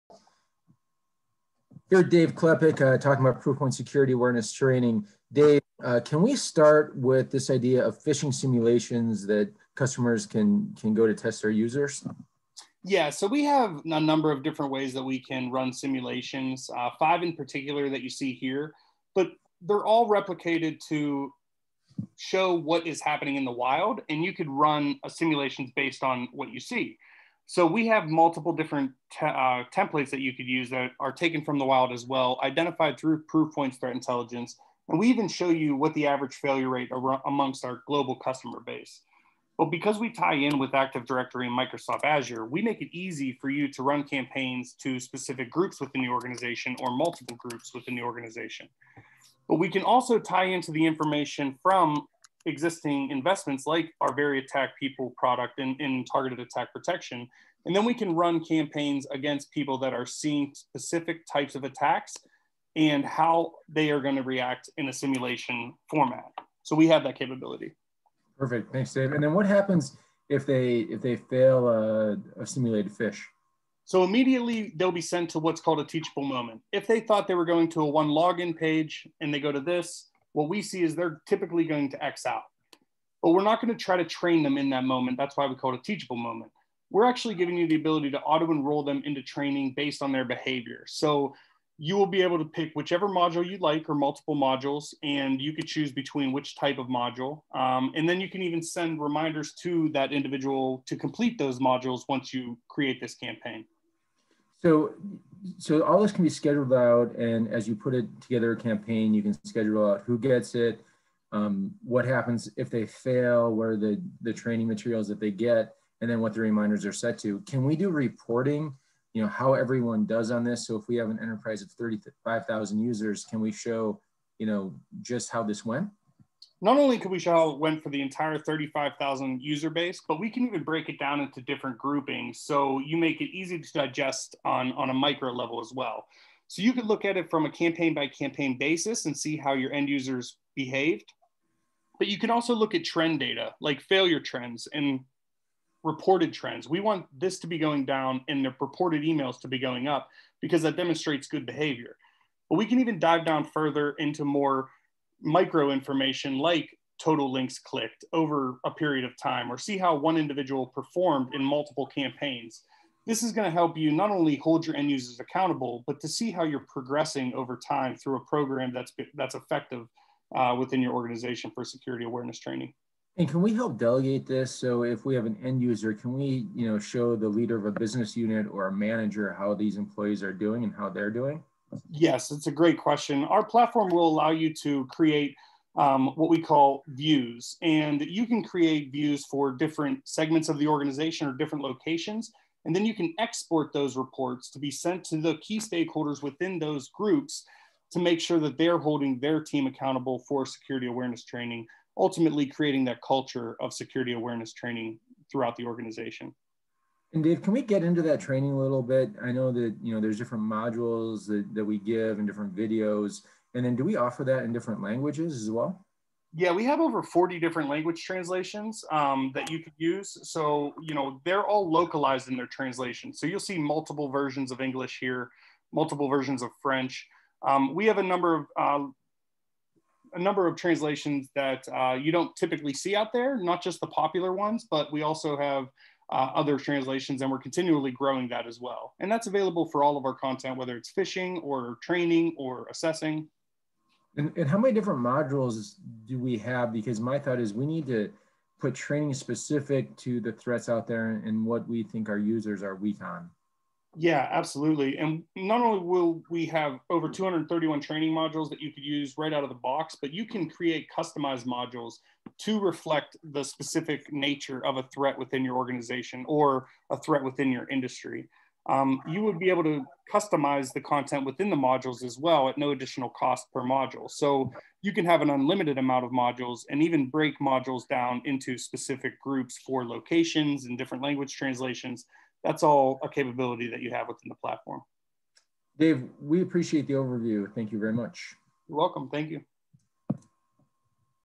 Proofpoint Security Awareness Short Overview
David Kleppick and Scott Rislov give a short overview of the Proofpoint Security Awareness Platform. We start with a Phishing Simulation Campaign, go to Reporting, and finish talking about the Training modules